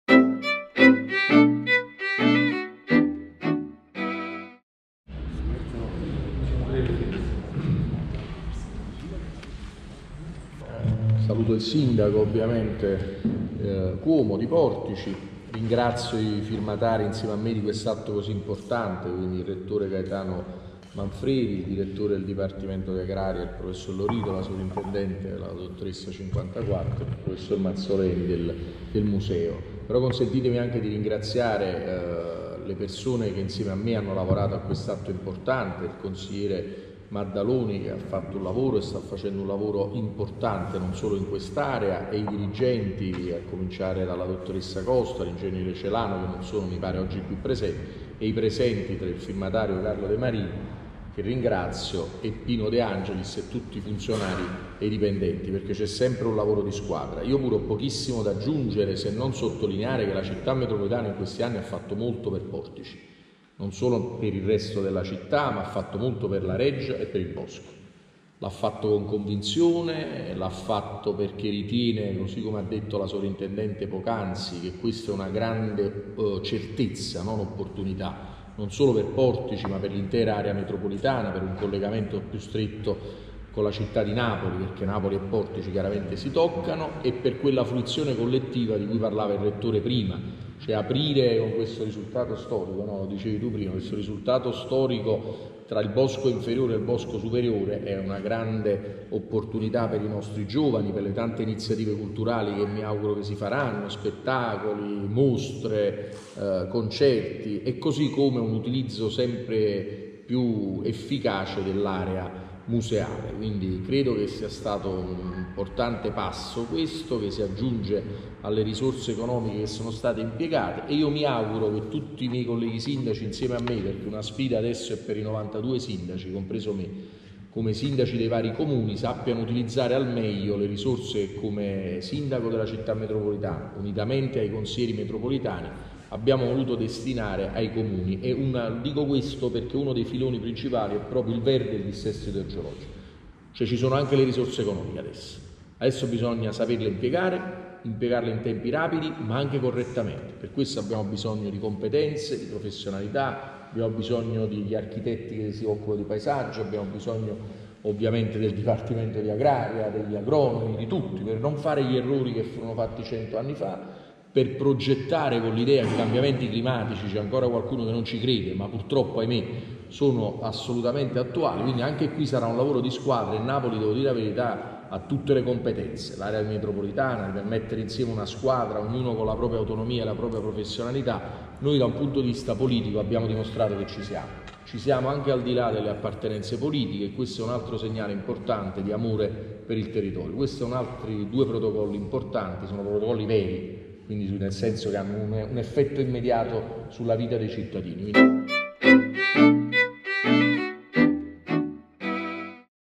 PORTICI L'INTERVENTO DEL SINDACO SINDACO
PORTICI: DOPO 150 ANNI IL BOSCO DIVENTA UN UNICA STRUTTURA. FIRMATI DUE IMPORTANTI ACCORDI TRA CITTA’ METROPOLITANA E UNIVERSITÀ’ DI NAPOLI. LA SODDISFAZIONE DEL SINDACO DE MAGISTRIS
Il Sindaco Metropolitano di Napoli, Luigi de Magistris ha firmato stamane a Portici, presso la Sala Cinese della Real Reggia, due convenzioni che consentiranno di poter gestire in maniera unitaria il Bosco di Portici dopo oltre 150 anni.